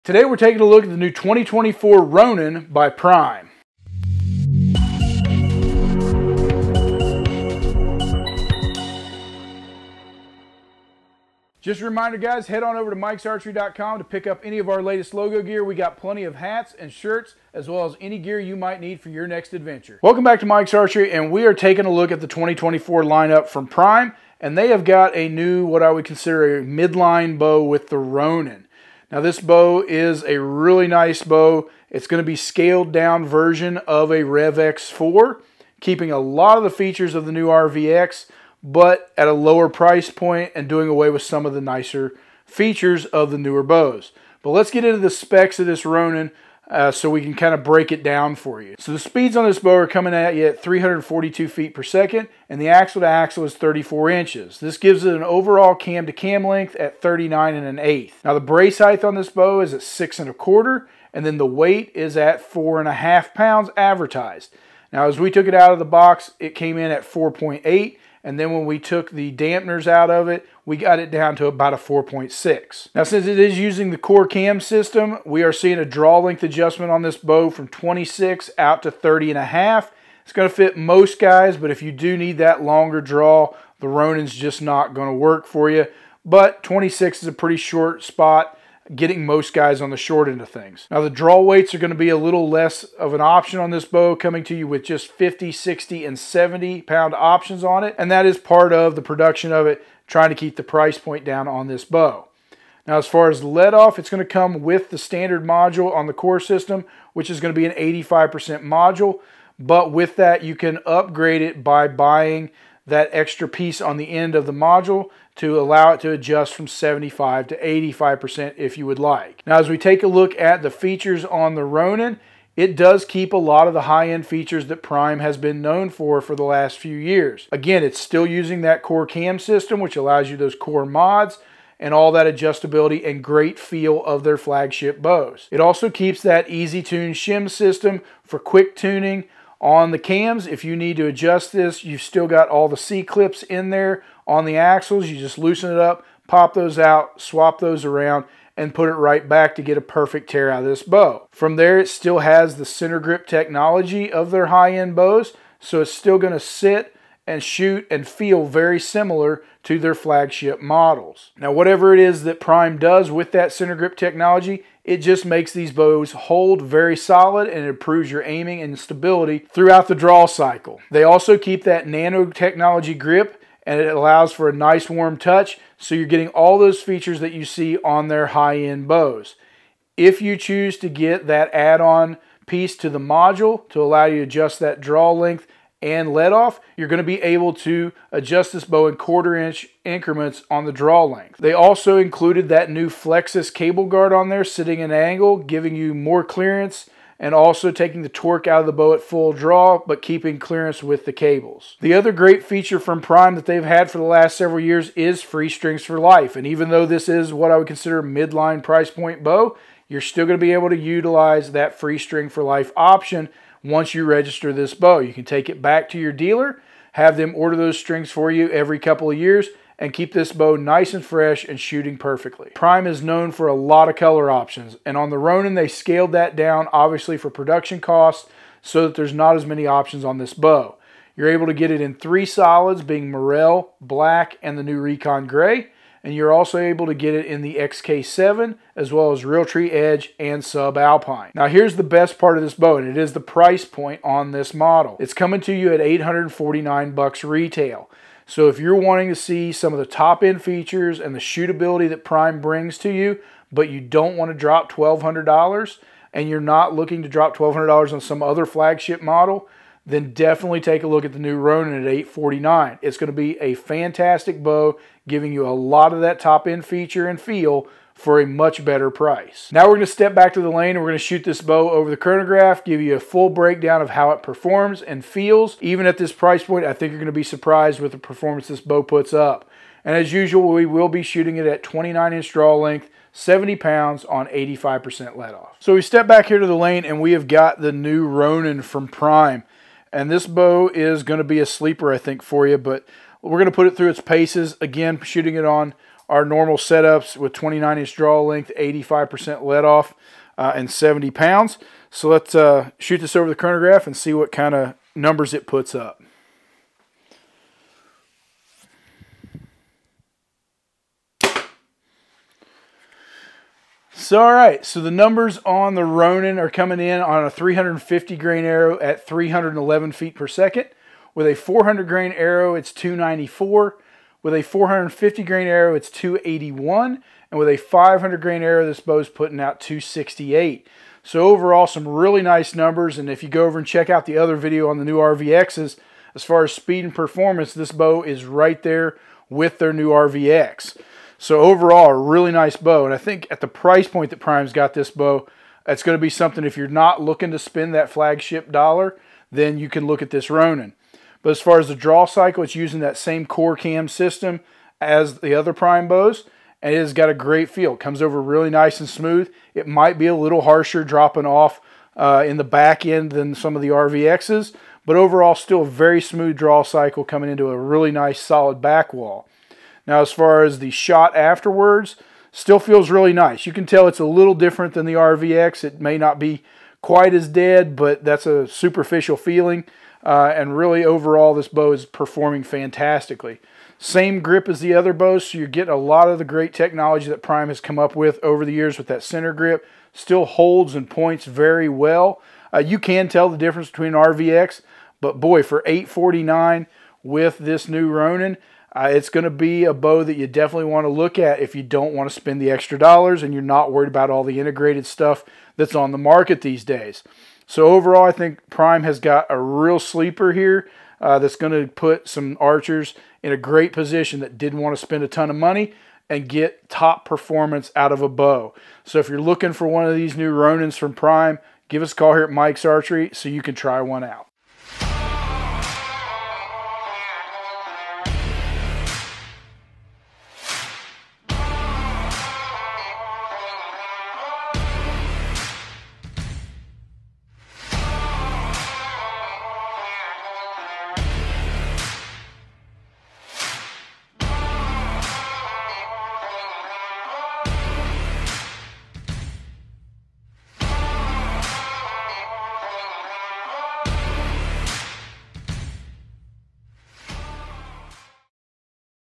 2024 Prime Ronan Bow Review: Get The FlagShip Feel Without Breaking The Bank!
We will be taking a look at the new 2024 Prime Archery Line up.  This video is a close look at the 2024 Prime Archery Ronan bow.

Prime Archery Ronan
Specs. $849.99 Bow Only MSRP
342 FPS
4.5 lbs Weight
6.25" Brace Height
34" Axle to Axle
Draw Length 26 1/2"-30 1/2"
75-85% Letoff on a Binary Cam
Available in 50lb, 60lb, 70lb max weights

00:00 Intro
00:29 Opening
00:45 Specs
01:10 Bow weight
01:24 Cam
02:33 Other Features
05:00 Color Options
06:00 Price
06:30 Shooting over Chrono
07:20 Noise Rating
07:30 Feel of Draw Cycle and shot